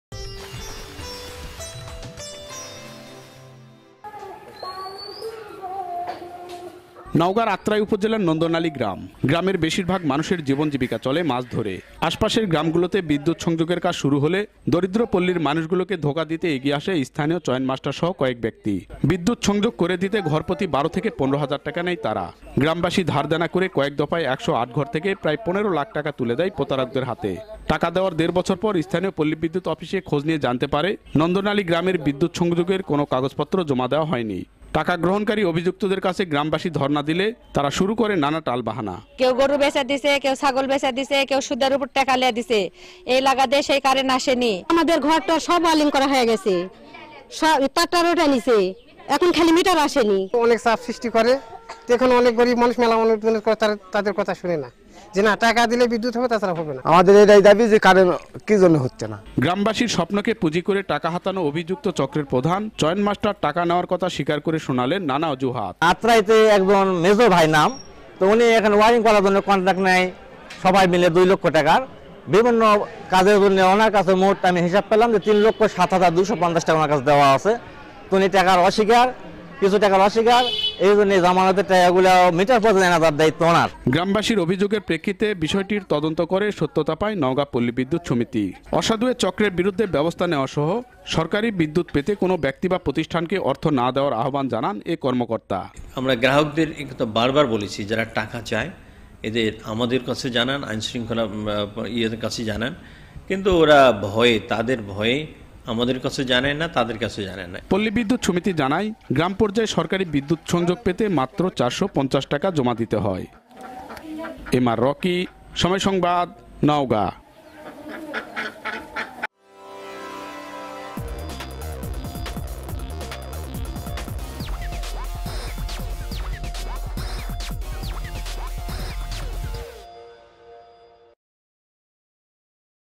তারা বলে টাকা দিলে বিদ্যুৎ আসবে তাছাড়া হবে না | Naogaon News
নওগাঁয় পল্লী বিদ্যুৎ সংযোগ দেয়ার নাম করে প্রায় ১৫ লাখ টাকা হাতিয়ে নিয়েছে একটি প্রতারক চক্র । বিদ্যুতের নতুন সংযোগ দিতে ঘর পিছু ১২ থেকে ১৫ হাজার টাকা করে নিয়েছে ঐ চক্রটি। টাকা দেয়ার প্রায় দেড় বছর অপেক্ষার পরও সংযোগ না পাওয়ায়, খোঁজ নিয়ে গ্রামবাসী জানতে পারে কোন কাগজপত্র জমা পড়েনি বিদ্যুৎ অফিসে। পল্লী বিদ্যুৎ সমিতি বলছে, অসাধু এ চক্রের সাথে বিদ্যুৎ বিভাগের কেউ জড়িত থাকলে কঠোর ব্যবস্থা নেয়া হবে ।

In Naogaon, a fraud cheater has taken Tk 15 lakh by giving the name of the connection of rural electricity. The group has taken Tk 12-15 thousand per house to give new connections to electricity. After waiting for about one and a half year to pay the money, without finding the connection, the villagers knew that no documents were deposited in the power office. The Palli Bidutut Samity said that strict action would be taken if anybody involved in the power sector is involved in dishonesty.